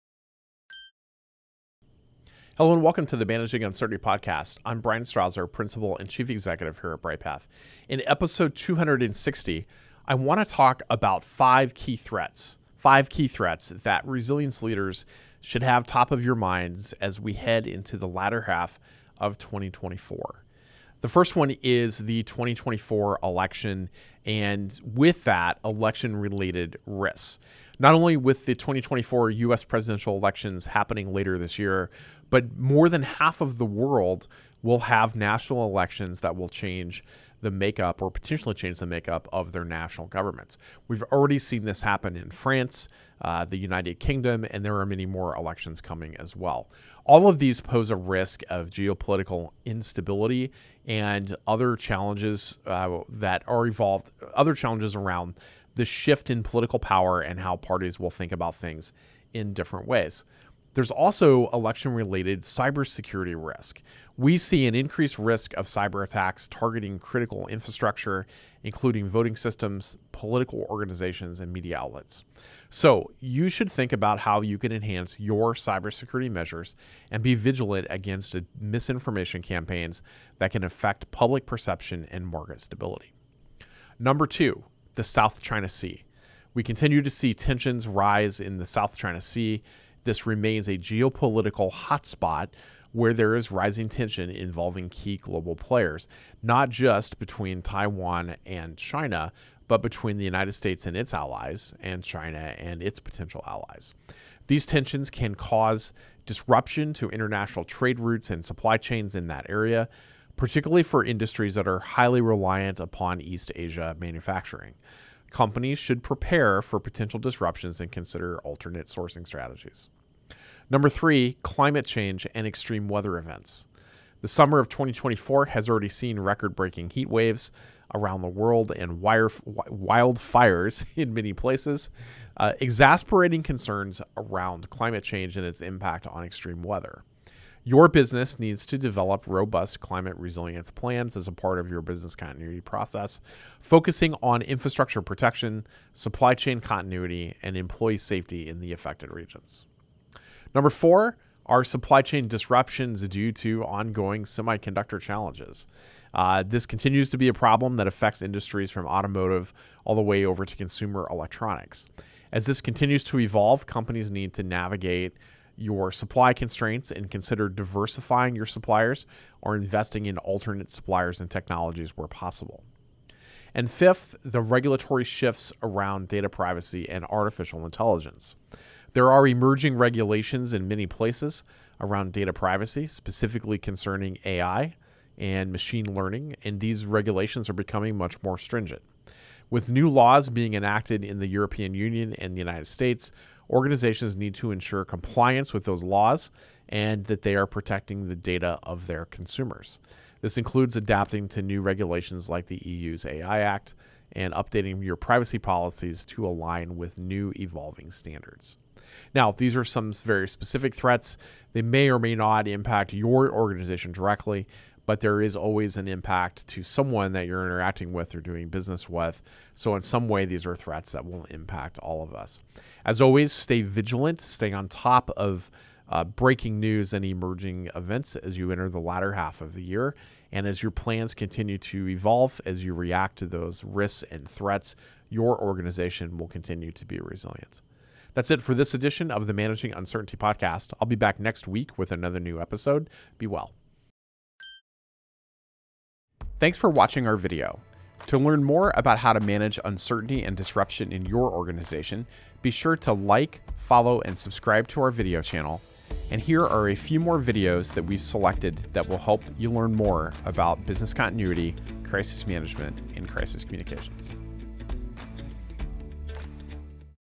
Episode #260: 5 Threats ahead in the latter half of 2024
In this episode of the Managing Uncertainty Podcast, Bryan Strawser, principal and Chief Executive at Bryghtpath, delves into five critical threats that resilience leaders need to prioritize as we approach the latter half of 2024. From the geopolitical upheavals related to the global election cycle to the rising tensions in the South China Sea, Bryan offers essential strategies to navigate these complex global landscapes. The episode also covers the severe impacts of climate change, supply chain disruptions, and the rapidly evolving regulatory environment for data privacy and AI.

Listeners will gain valuable insights into developing robust climate resilience plans, diversifying suppliers in the semiconductor industry, and ensuring compliance with stringent new data privacy laws. Whether it's preparing for potential disruptions to international trade routes or enhancing cybersecurity measures to guard against election-related cyber attacks, this episode provides a comprehensive roadmap for maintaining business continuity amidst uncertainty. Tune in to equip yourself with the knowledge and strategies needed to keep your organization resilient and ready for whatever comes next.